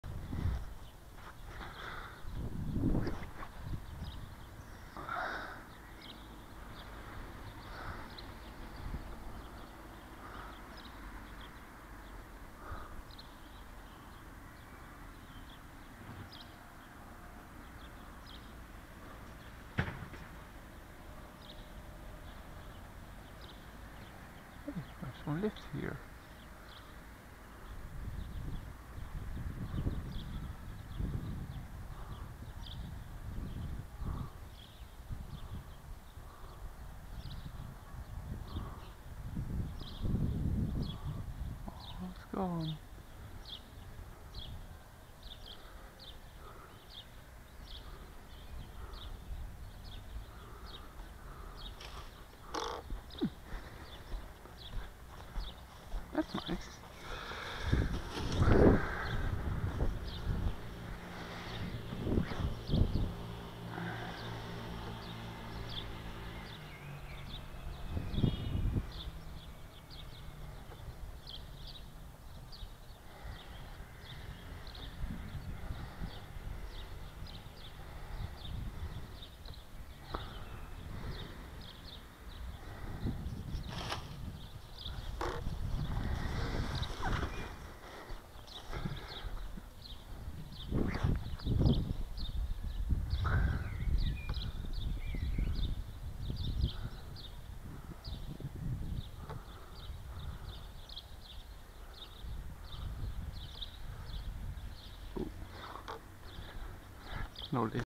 Bird plane balsa DLG first thermal
Caught my first little thermal when the sun partly pierced the clouds. It gained a bit of altitude and extended the flight to 50 seconds,  Later partly caught another for 30seconds of flight.
Addictive fun,,,,,  i want some more  :)